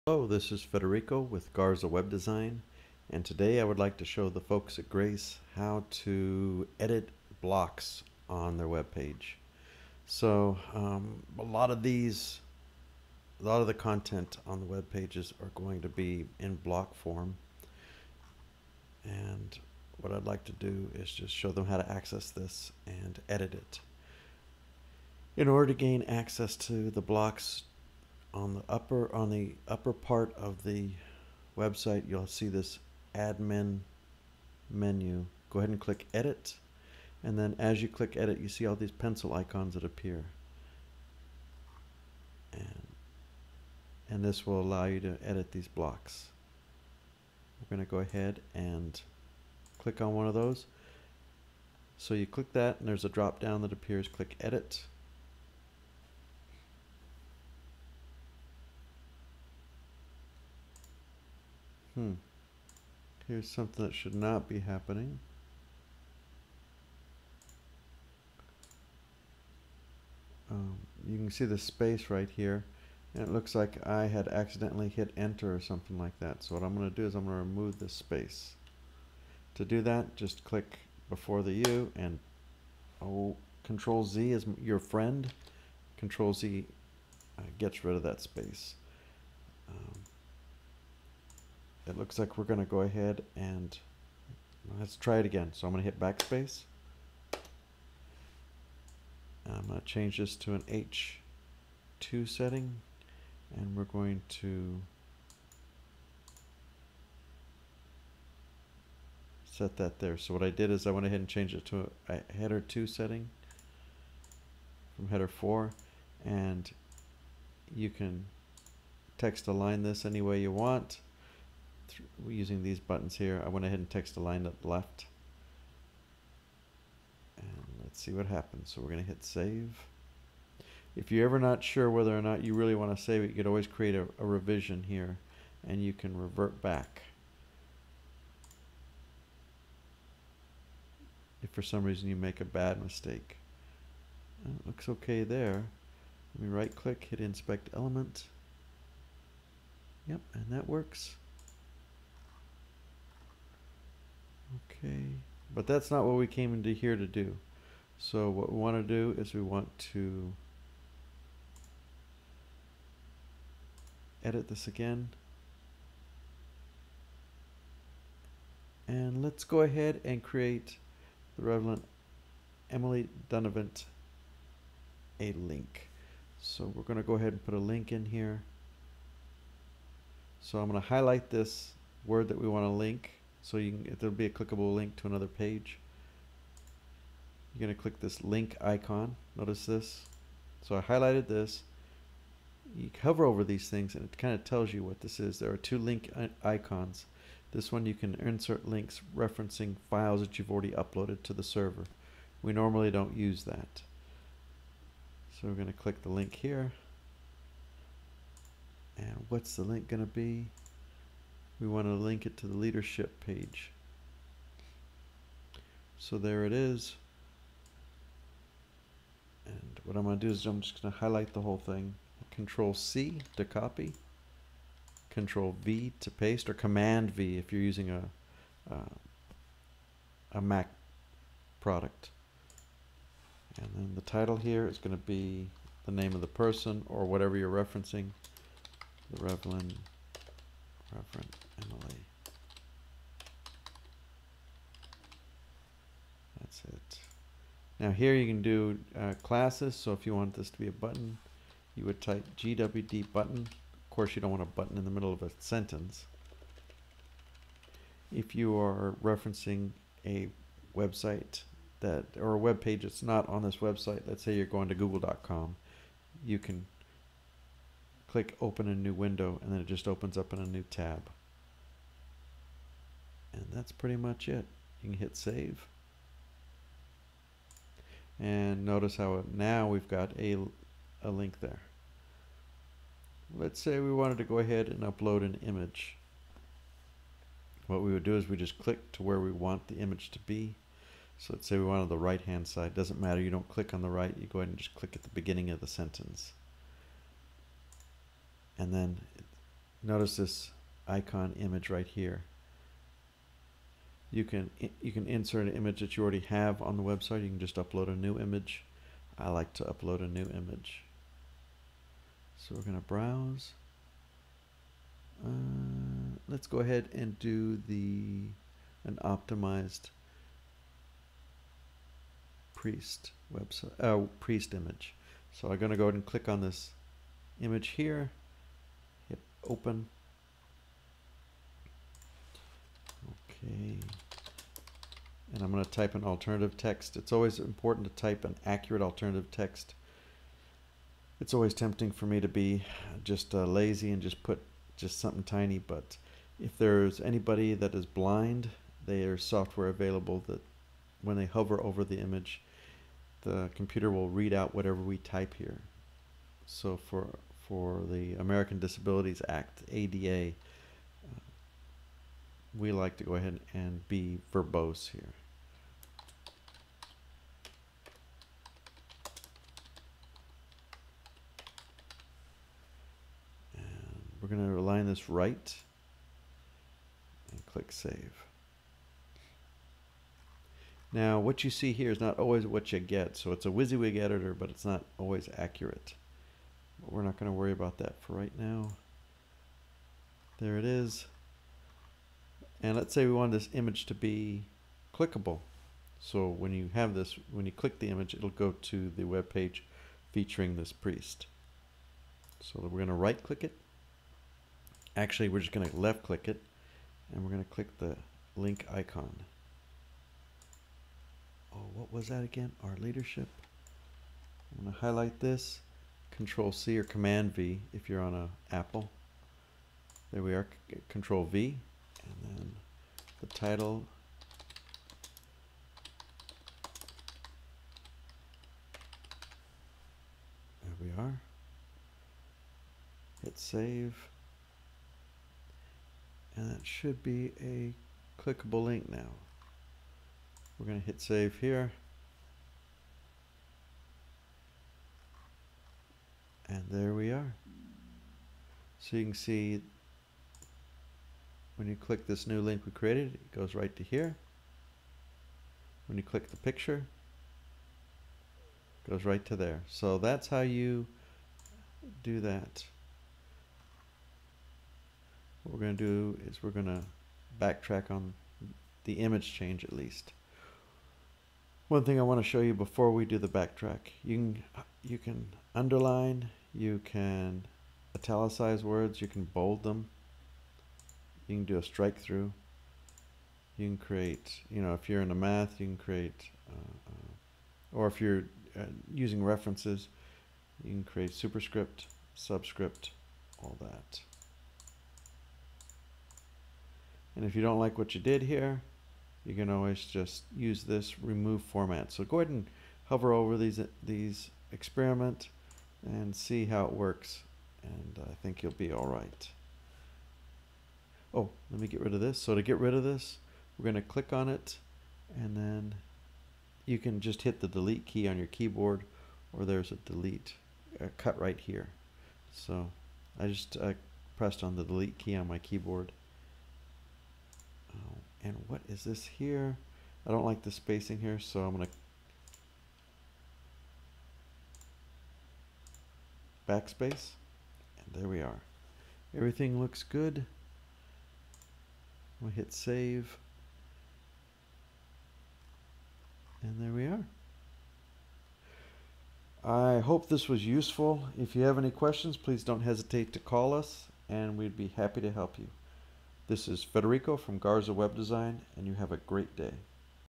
How to Edit a Block in Drupal 8
This is a simple tutorial on how to edit a block.  Included in this tutorial is how to add an image and float it as well as how to add attach a link to text.